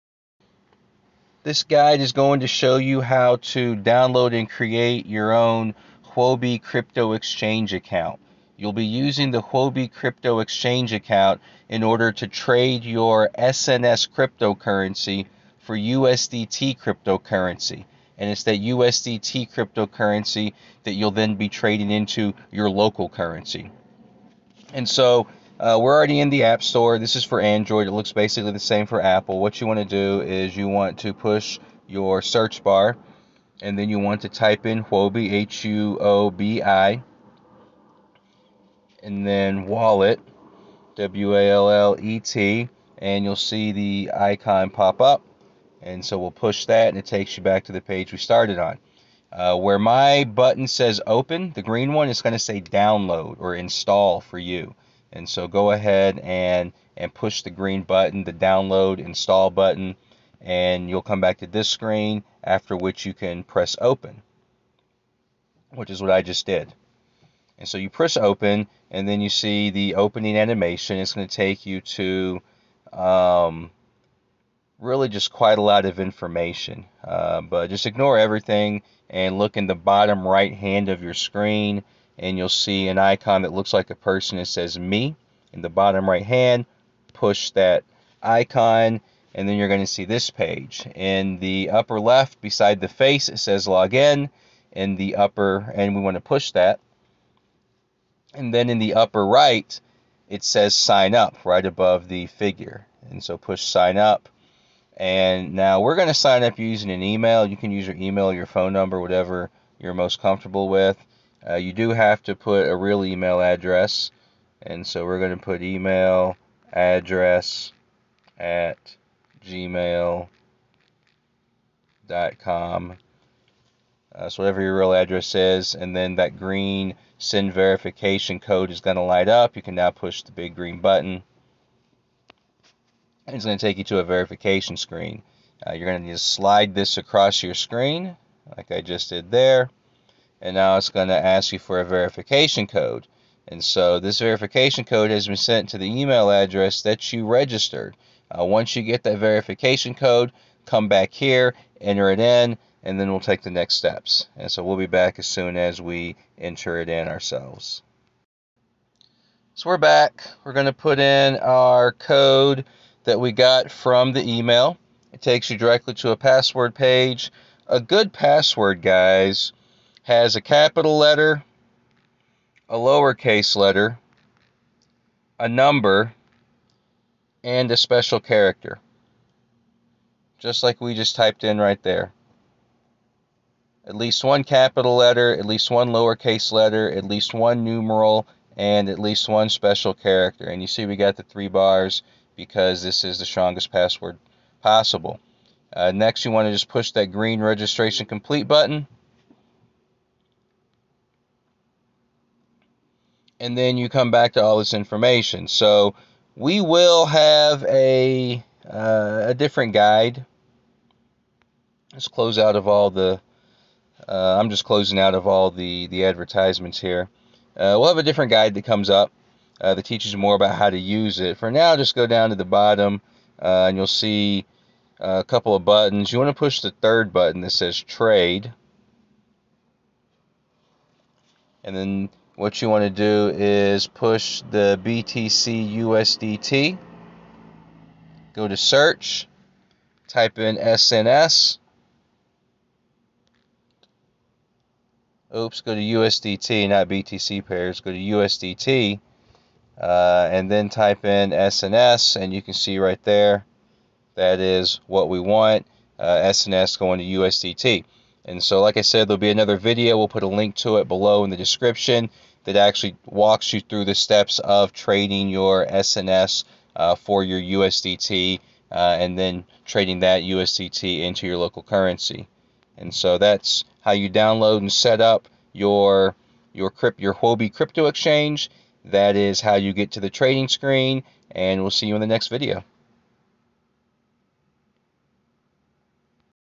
How to Set Up a Huobi Global Crypto Exchange Account
Step by step guide for setting up Huobi Global crypto exchange account.